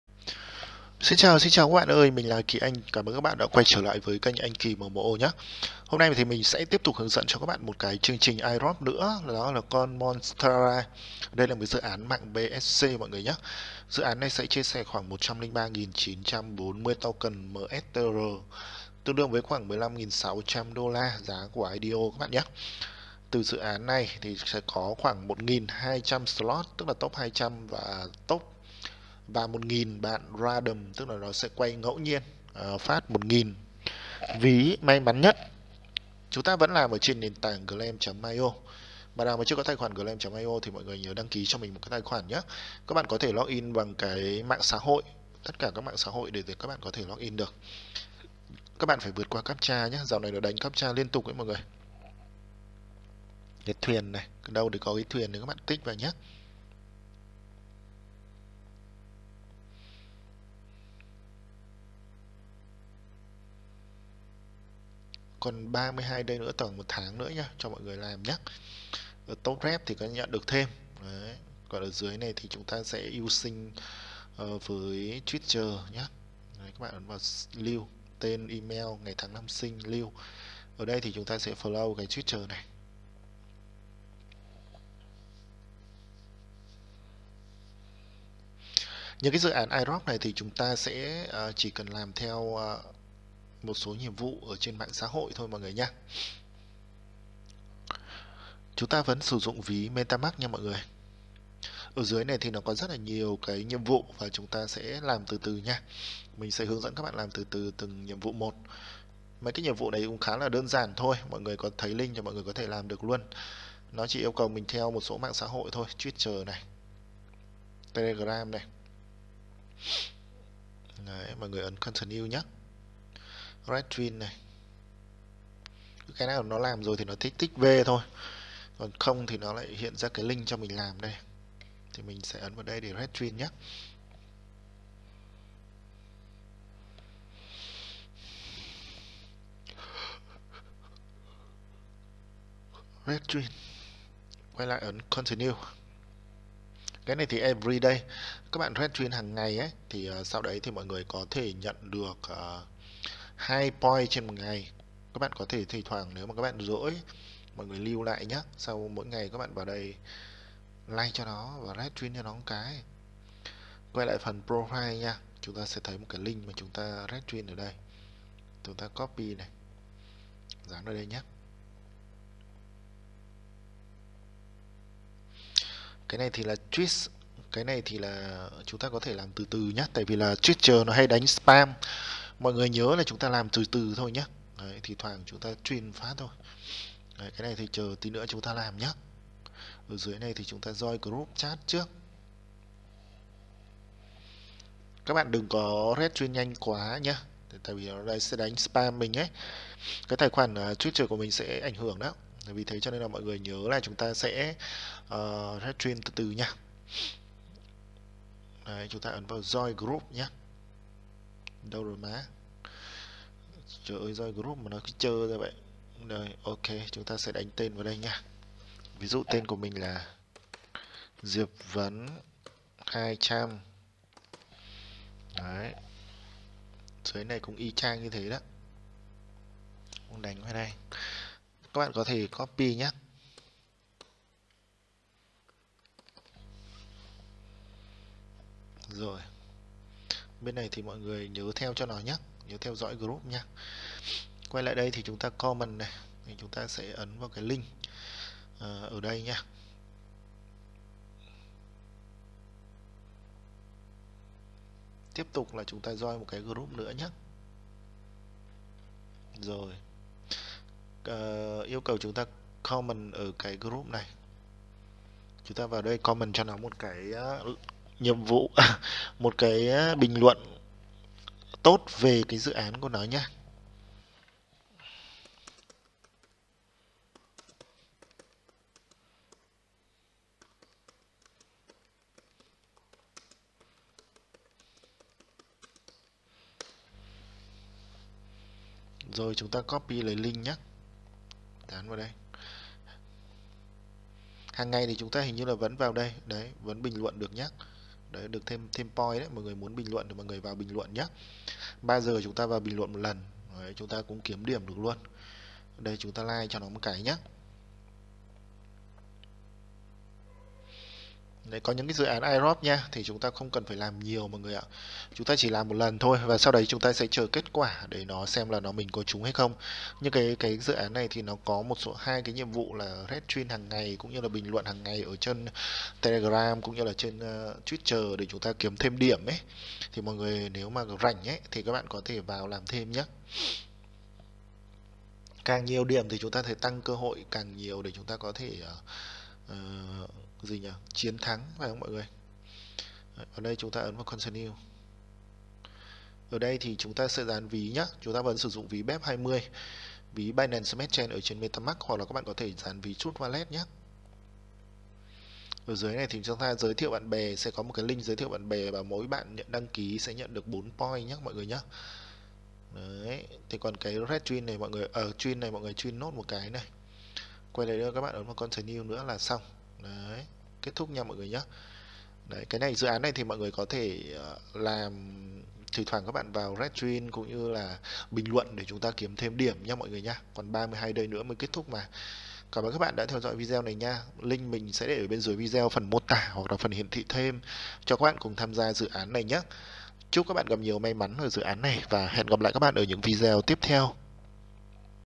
Kèo ăn chia 103.940 token MSTR ~ 15.600$ (giá IDO) từ dự án game Monsterra giới hạn 1200 slot
🎁Kèo ăn chia 103.940 token MSTR ~ 15.600$ (giá IDO) từ dự án game Monsterra giới hạn 1200 slot (200 top + 1000 random)